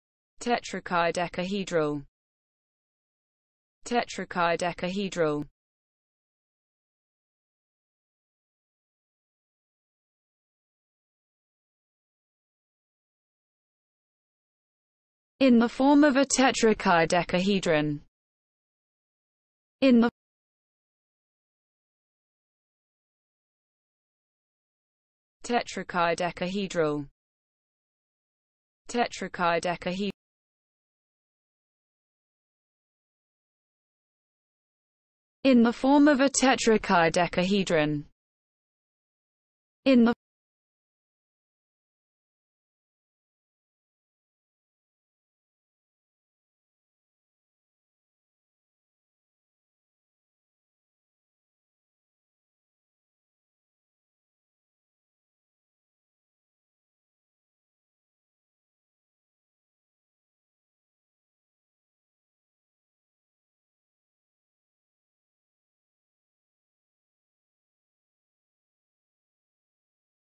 What does Tetrakaidekahedral mean?
What does Tetrakaidekahedral mean in English? Learn the meaning of the word Tetrakaidekahedral! How to Pronounce Tetrakaidekahedral. Expand your vocabulary, learn words.